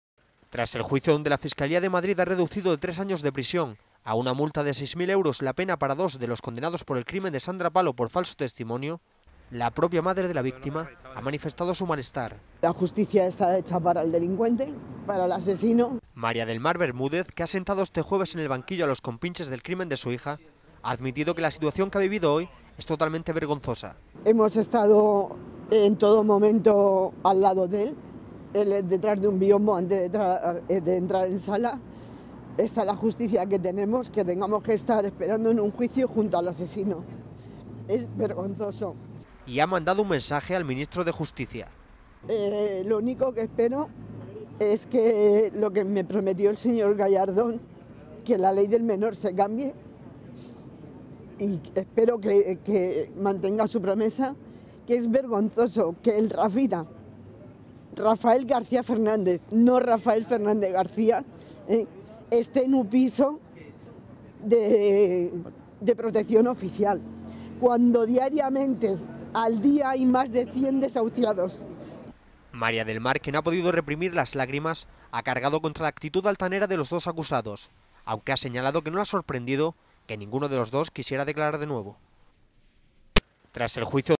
Bermúdez: "La Justicia está hecha para el asesino"
Tras el juicio donde la Fiscalía de Madrid ha reducido de tres años de prisión a una multa de 6.000 euros la pena para dos de los condenados por el crimen de Sandra Palo por falso testimonio , la propia madre de la víctima, María del Mar Bermúdez, ha manifestado su malestar, ya que según ellla, la Justicia "está hecha para el asesino".